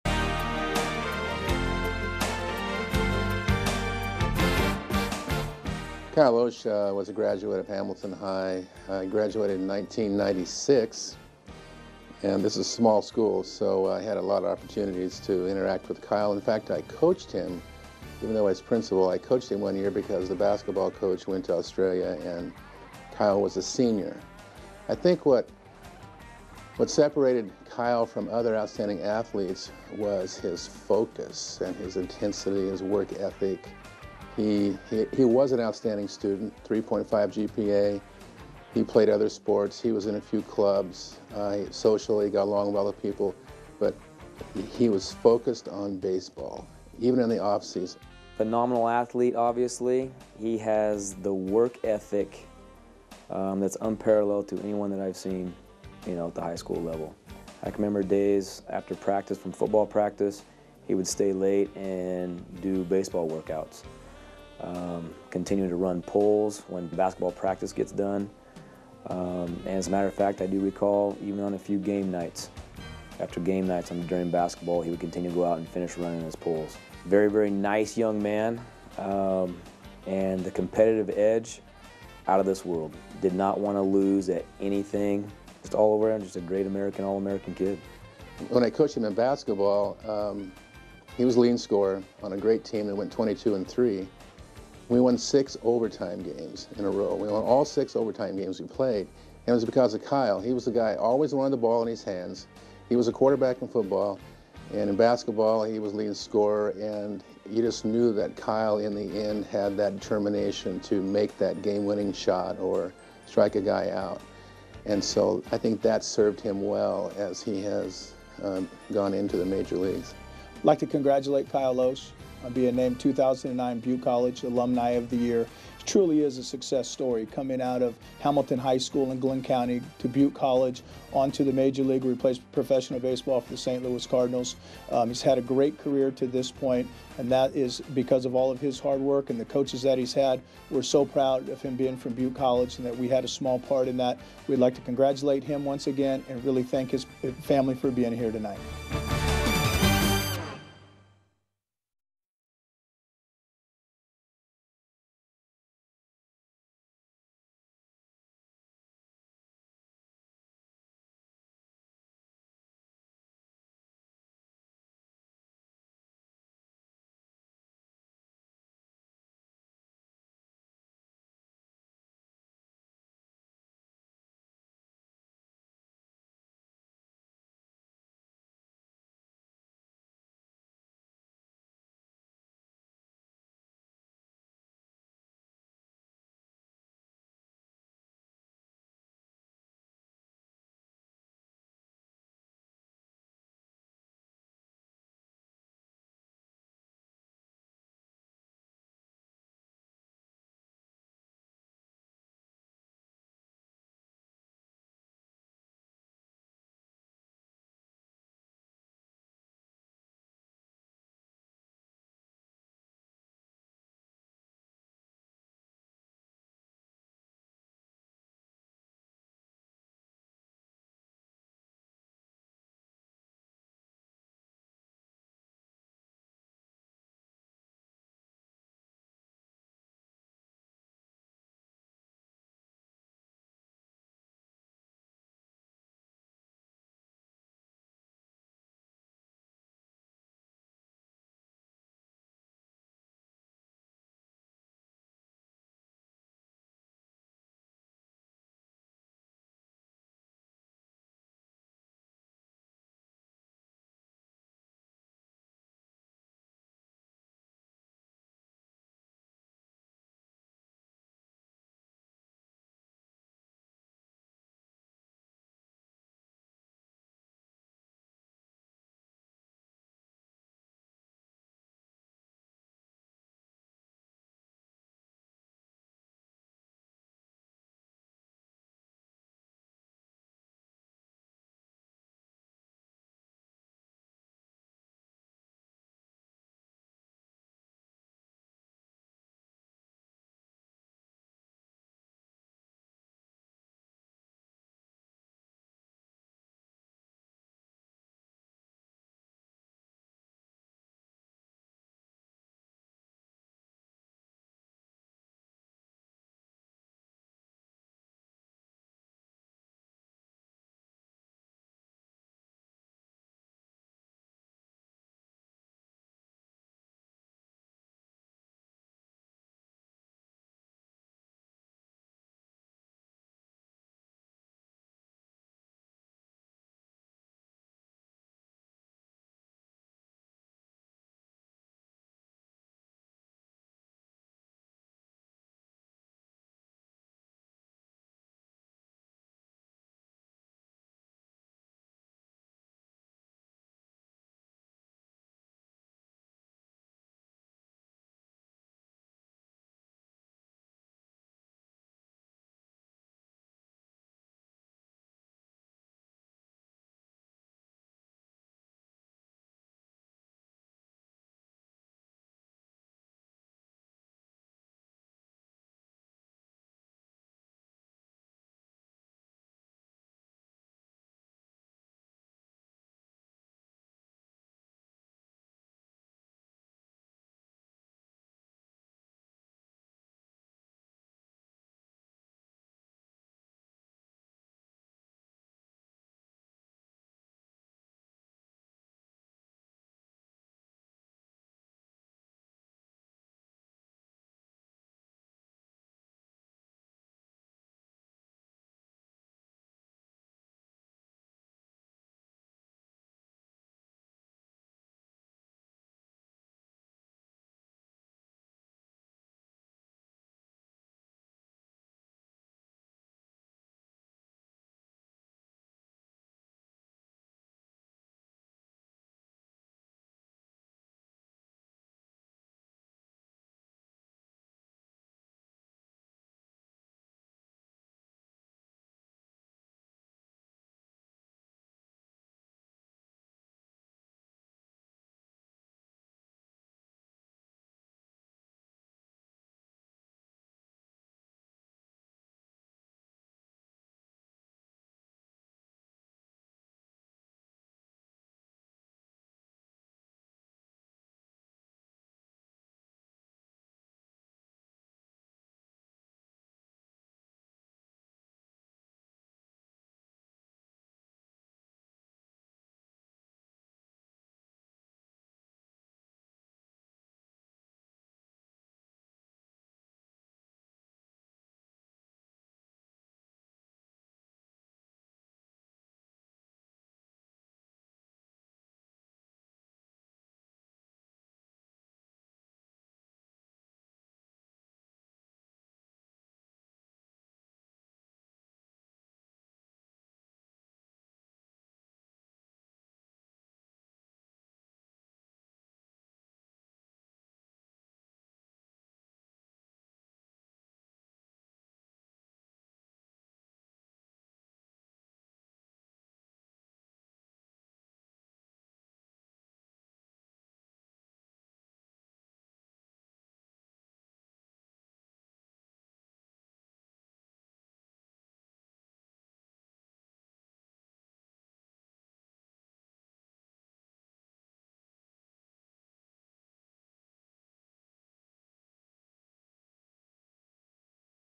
Kyle Lohse, 2008 Butte College Outstanding Alumnus
Kyle Lohse, starting pitcher for the St. Louis Cardinals, is awarded the Butte College 2008 Outstanding Alumnus award at the Butte College Foundation's annual Spring Gala.